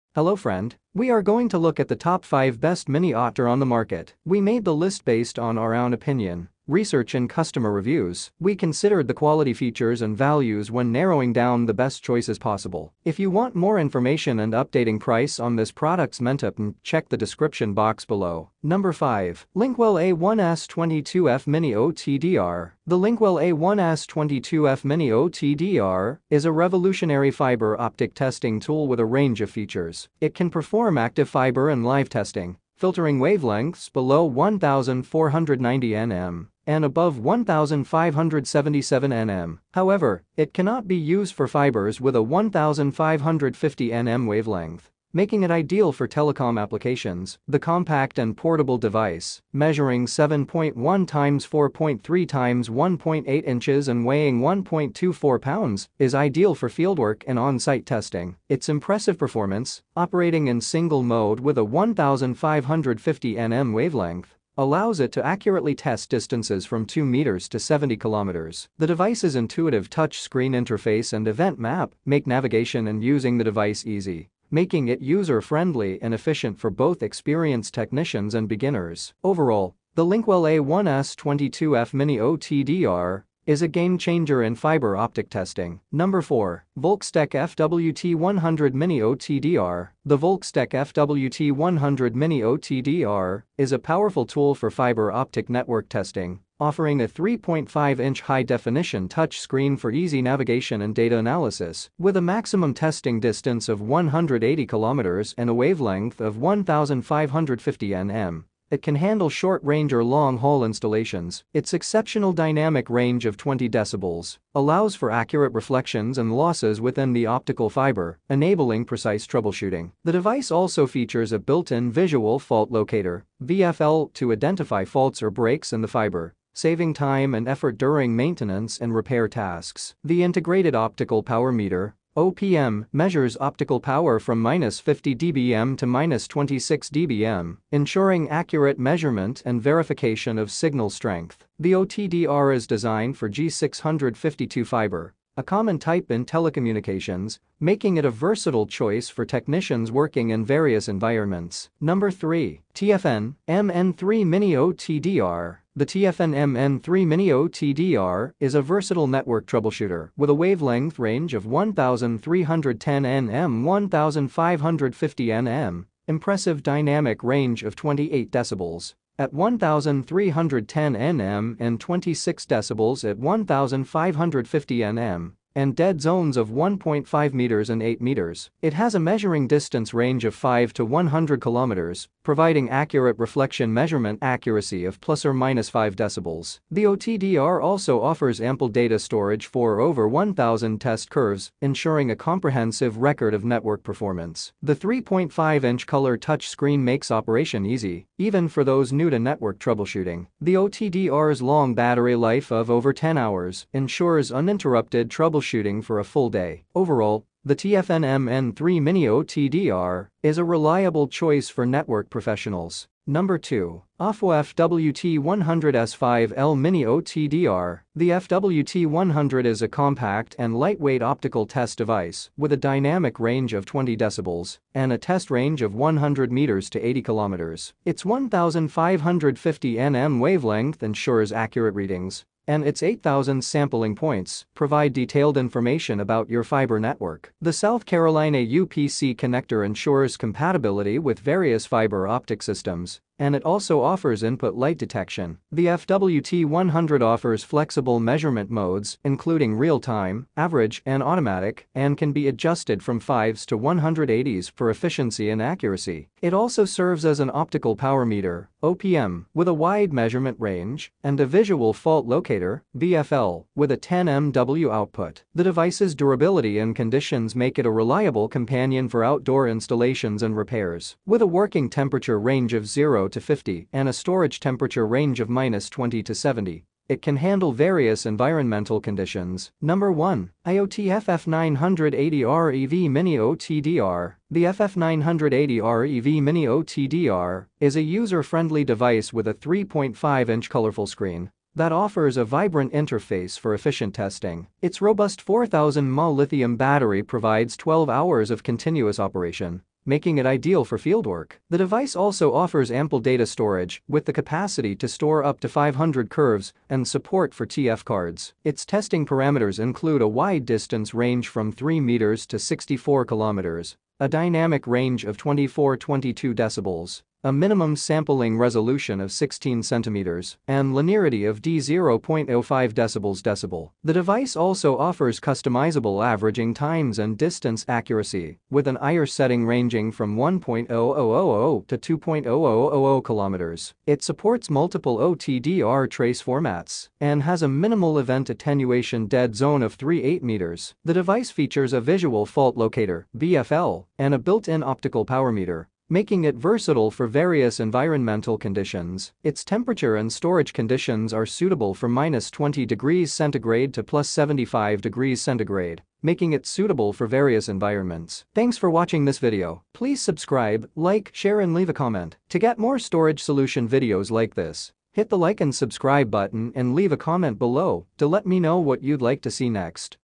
TOP 5 Best Mini OTDR Review in 2024
TOP 5 Best Mini OTDR Review in 2025. In this video, we'll showcase the latest and greatest Mini OTDR devices that every technician and engineer should consider. These powerful tools are essential for testing and troubleshooting optical fiber networks.

LinkWell A1-S22F MINI OTDR

VOLKSTEC FWT-100 MINI OTDR

TFN MN3 MINI OTDR

OFW FWT-100-S5L MINI OTDR

IOT FF-980REV MINI OTDR